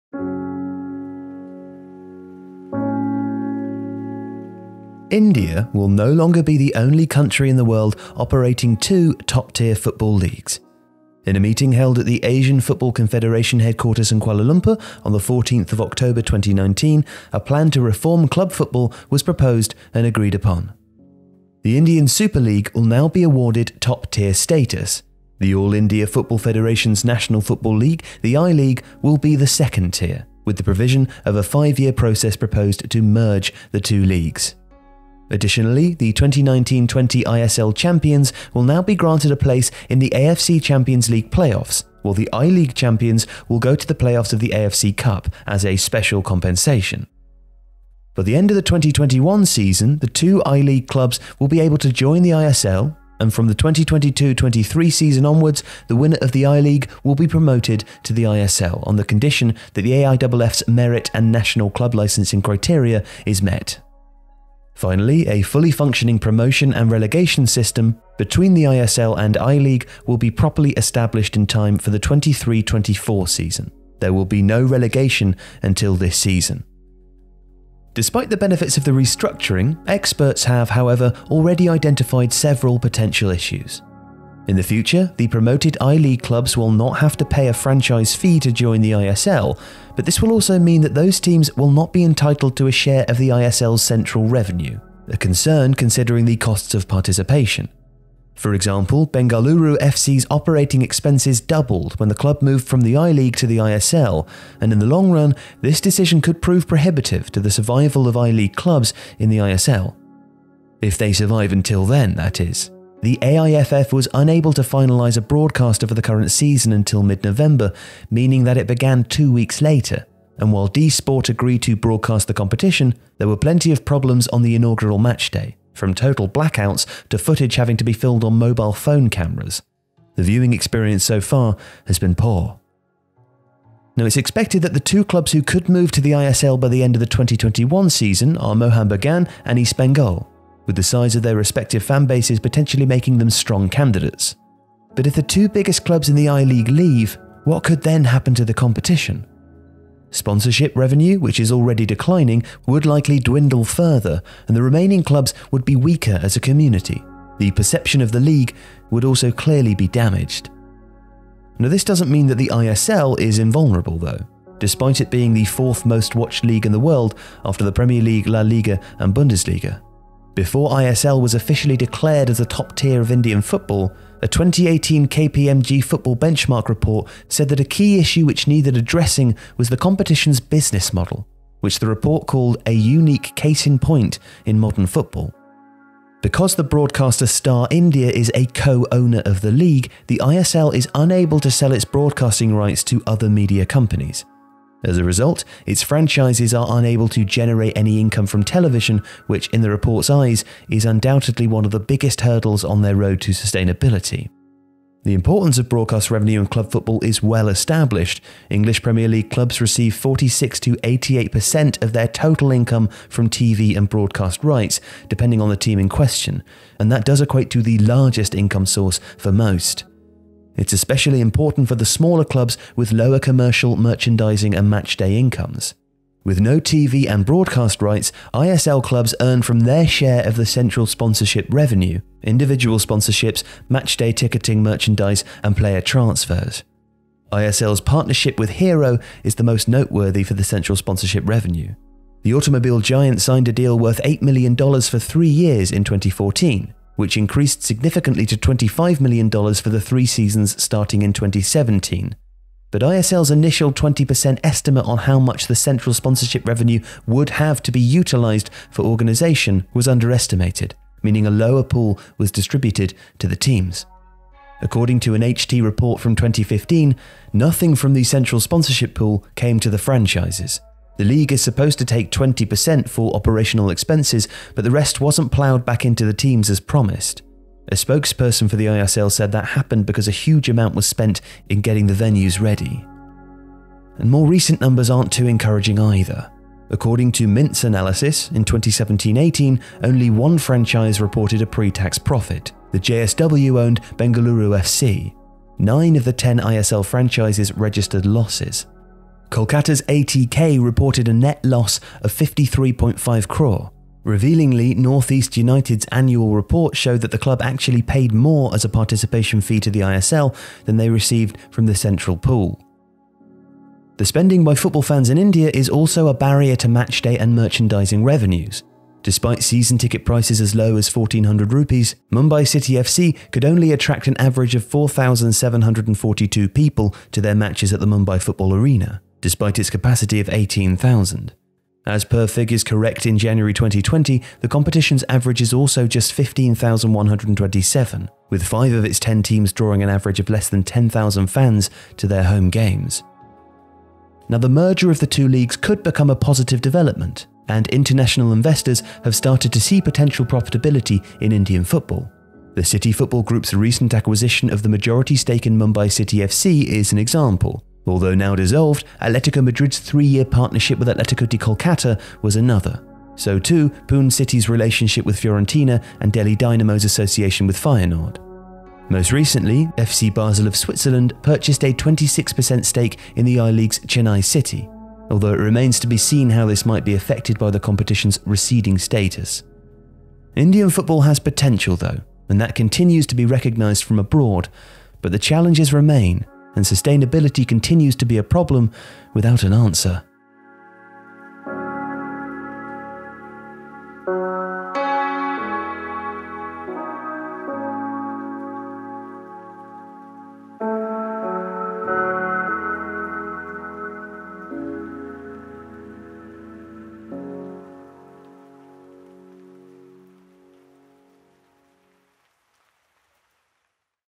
India’s Football Merger Has Some Problems
India will no longer be the only country in the world operating two top-tier football leagues. In a meeting held at the Asian Football Confederation headquarters in Kuala Lumpur, on 14 October 2019, a plan to reform club football was proposed and agreed upon.

The Indian Super League will now be awarded top-tier status. The All India Football Federation’s national football league, the I-League, will be the second tier, with the provision of a five-year process proposed to 'merge' the two leagues.

Despite the benefits of the restructuring, experts have, however, already identified several potential issues.